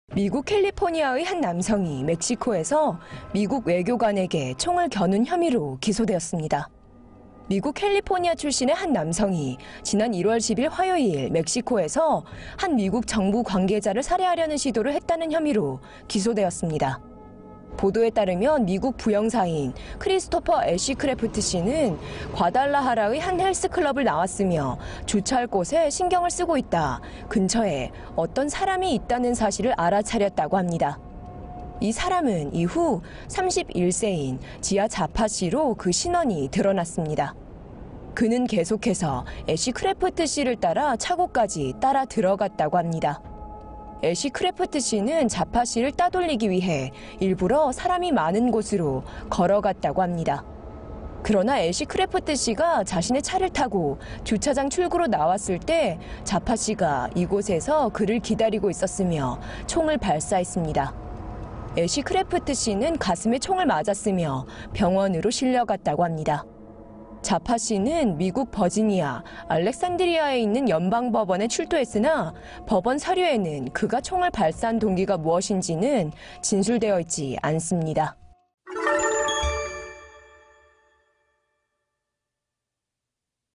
한 남성, 멕시코에서 미국 외교관을 향해 총 발사한 혐의로 기소
미국, 멕시코, 과달라하라 — 미국, 캘리포니아의 한 남성이 멕시코에서 미국의 외교관에게 총을 겨눈 혐의로 기소되었습니다.

미국, 캘리포니아 출신의 한 남성이 지난 1월 10일 화요일 멕시코에서 한 미국 정부 관계자를 살해하려는 시도를 했다는 혐의로 기소되었습니다.

보도에 따르면 미국 부영사인 크리스토퍼 애쉬크래프트 씨는 과달라하라의 한 헬스클럽을 나왔고, 주차할 곳에 신경을 쓰고 있다, 근처에 어떤 사람이 있다는 사실을 알아차렸다고 합니다.

이 사람은 이후 31세인 지아 자파 씨로 그 신원이 드러났습니다. 그는 계속해서 애쉬크래프트 씨를 따라 차고까지 따라들어갔다고 합니다. 애쉬크래프트 씨는 자파 씨를 따돌리기 위해, 일부러 사람이 많은 곳으로 걸어갔다고 합니다.

그러나 애쉬크래프트 씨가 자신의 차를 타고 주차장 출구로 나왔을 때, 자파 씨가 이 곳에서 그를 기다리고 있었으며, 총을 발사했습니다.

애쉬크래프트 씨는 가슴에 총을 맞았고, 병원으로 실려갔다고 합니다.

자파 씨는 미국, 버지니아, 알렉산드리아에 있는 연방법원에 출두했으나, 법원서류에는 그가 총을 발사한 동기가 무엇인지는 진술되어있지 않습니다.